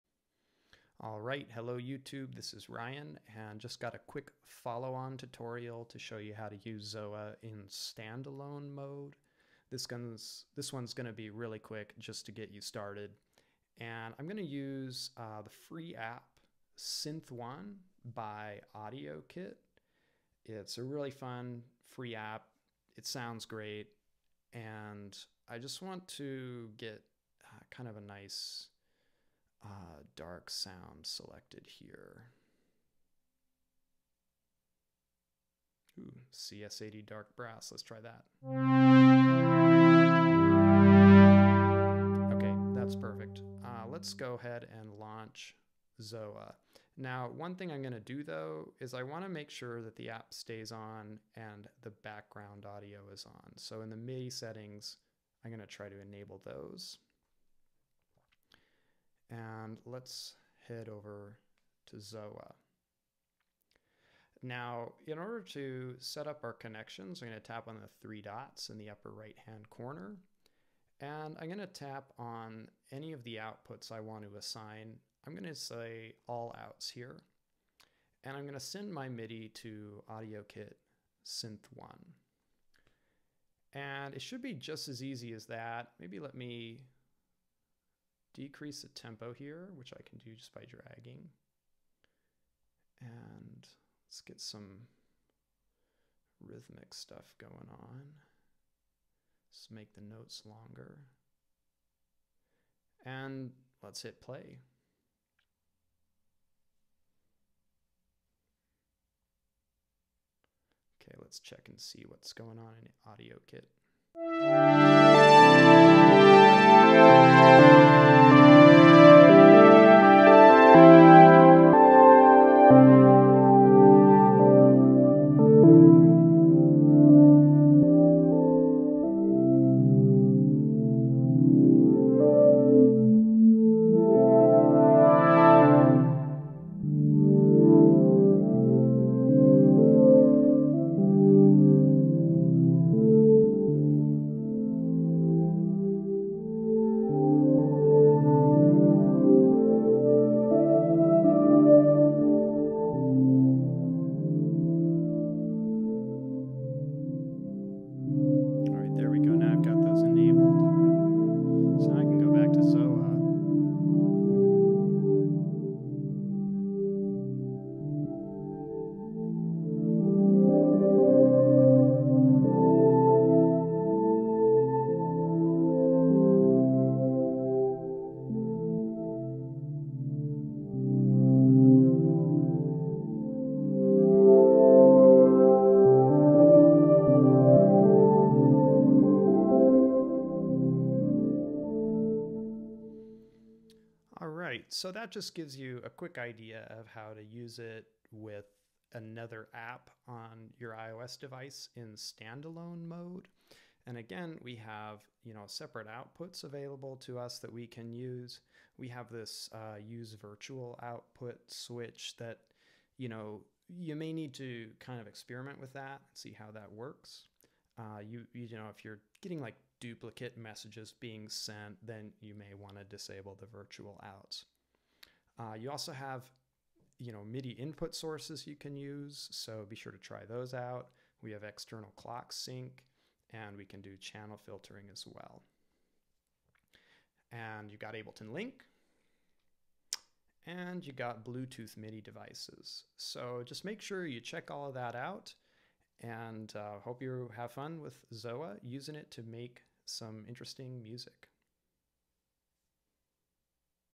ZOA Tutorial 08 — Standalone Mode
ZOA is a creative MIDI sequencer for iOS and macOS that implements Conway’s Game of Life. In this series of videos, I give an overview of how to get the most out of ZOA in AUM.

ZOA is a MIDI sequencer, so it does not generate sound on its own. Instead, ZOA sends messages to other apps and plugin that tell them what notes to play. ZOA can run as a standalone app or as an AUv3 plugin.